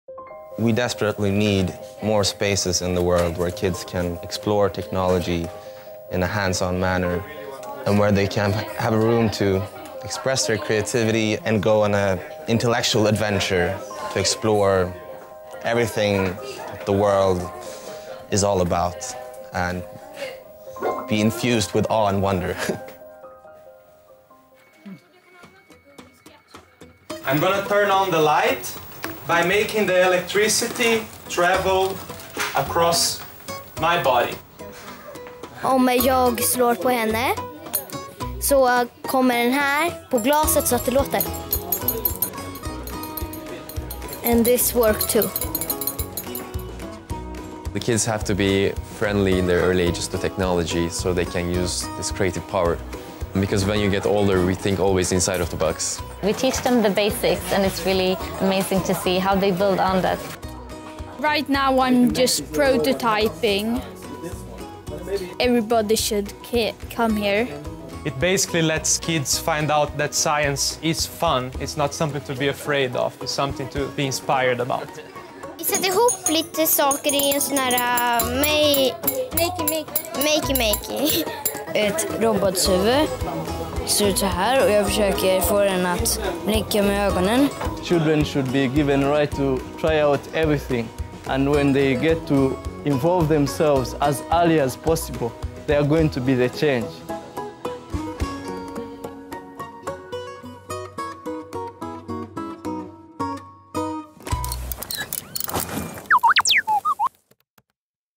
Kids Hack Day Kickoff Event - Stockholm, Sweden
This footage is based on the very first Kids Hack Day event that took place on August 31st, 2013, in Stockholm, Sweden.

Kids Hack Day is a global movement that organizes several one-off days of immersive technology-related activities for kids. The goal is to supplement traditional education curriculums worldwide with these hack days. The hack days are intended to inspire creativity and provide a hands-on approach to learning about technology, which we believe is lacking in schools.

Revolutionizing children's education, 1 hack day at a time.

Are you interested in organizing a Kids Hack Day event in your city?

Video Production by Flip-Flop Interactive

Society46, Sparkfun, Creatables, Bare Conductive, The3DClassroom, 3DVerkstan, Stockholm Makerspace, Skatehack, Sensavis, Hermans Tradgard, La Tamponneuse, Eventopia